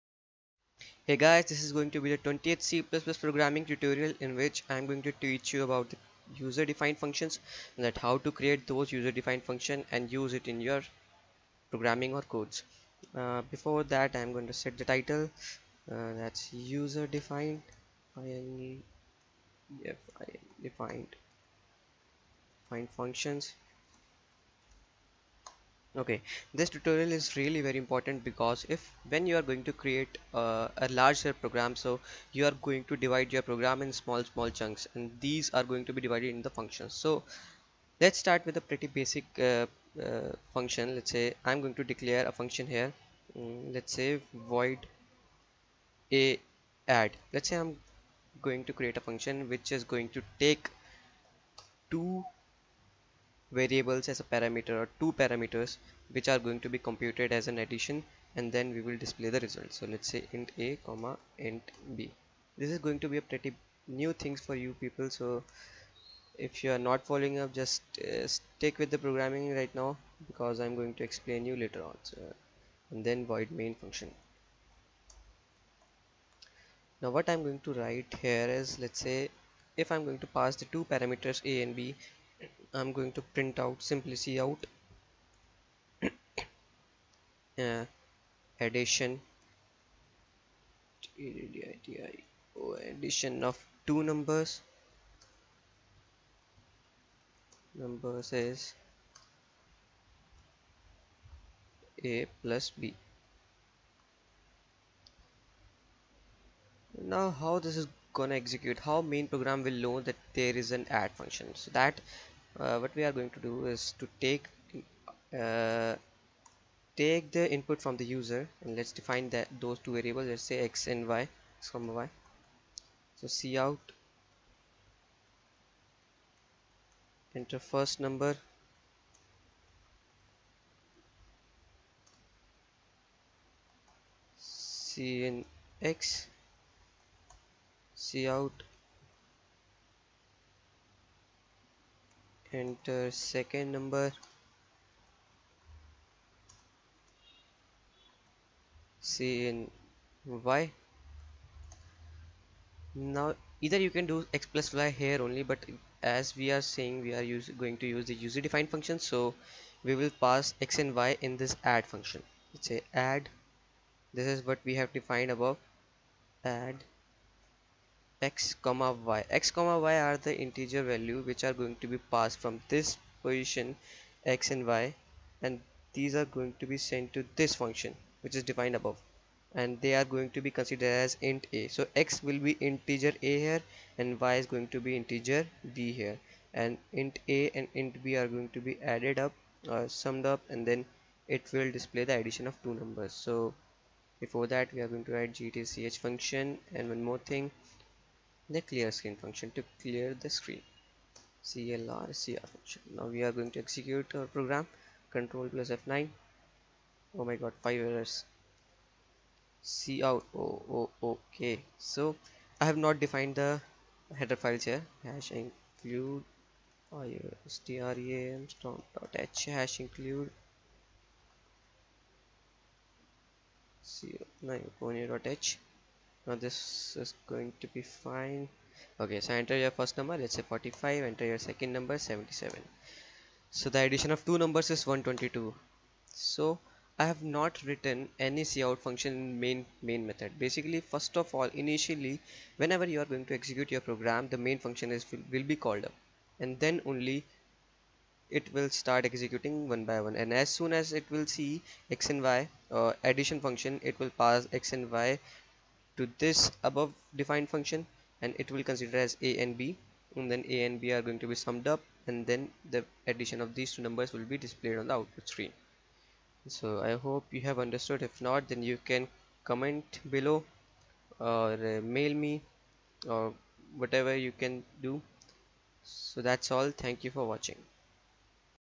C/C++ tutorials for beginners on Turbo C++ Tutorial 20 : User defined functions in c++
In this c++ tutorial you will learn about user defined functions and how to create those user defined functions.